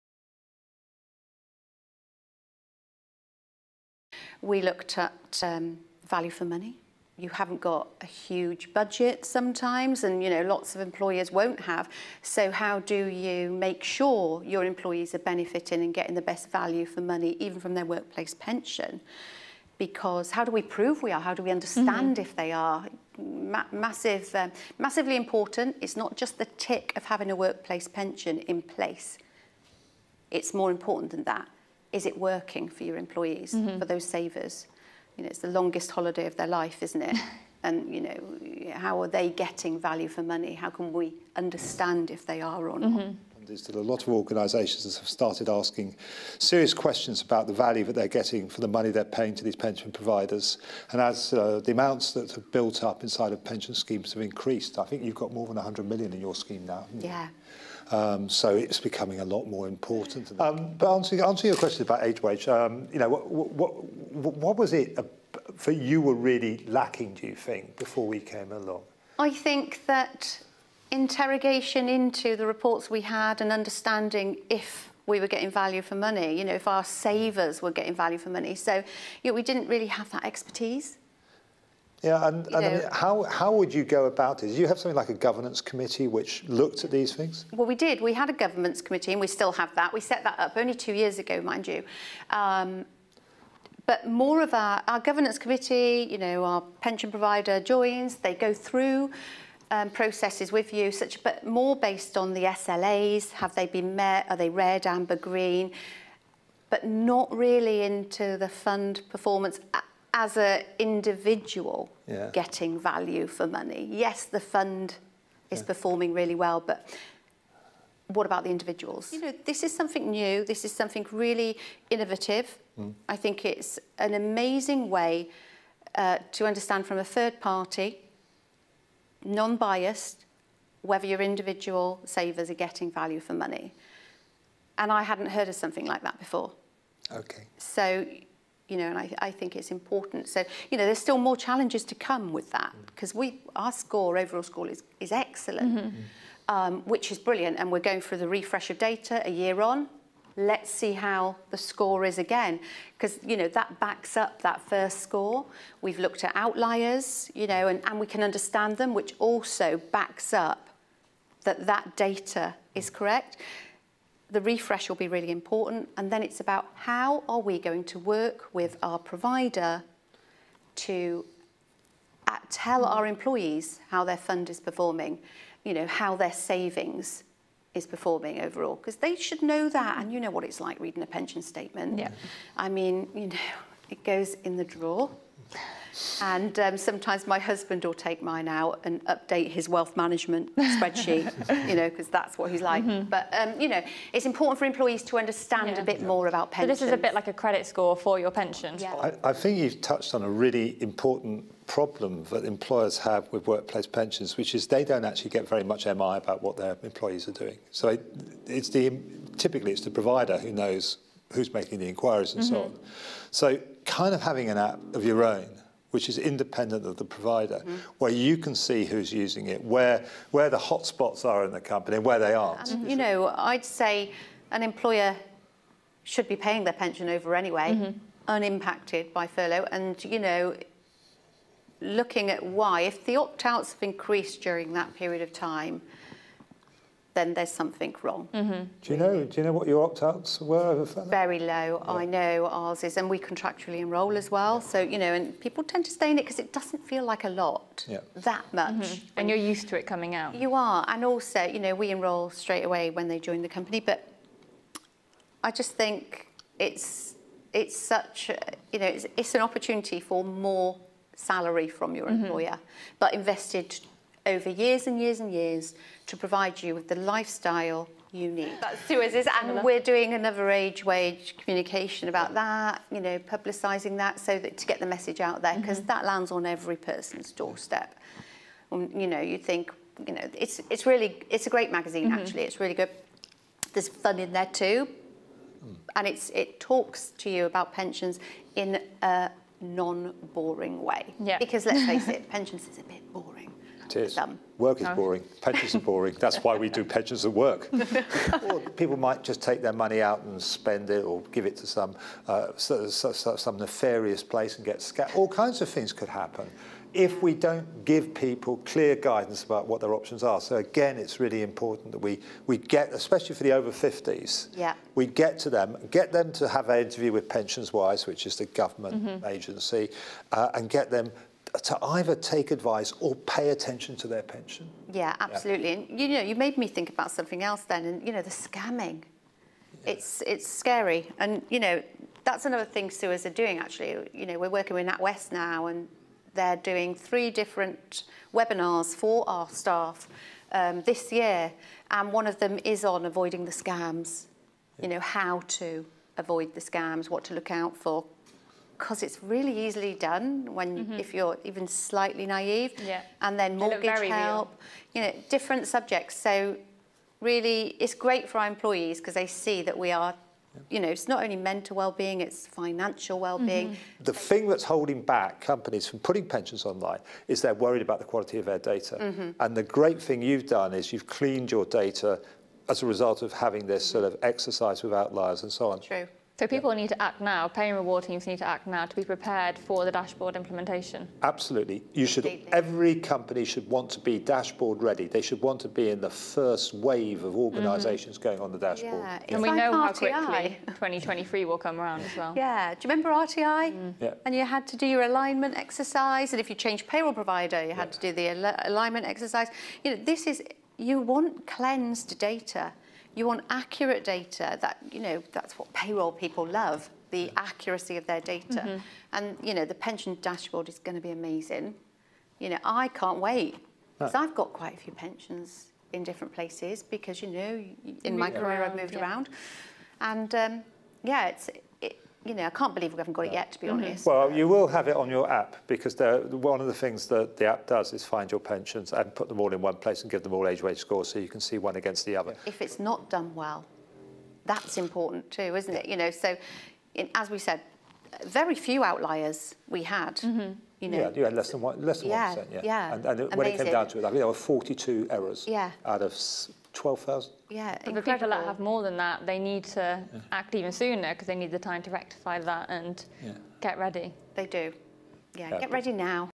How SUEZ , Aegon and AgeWage are working together
Edited highlights of a webinar filmed by Payroll and Reward on how employers can be progressive in promoting their workplace pensions to staff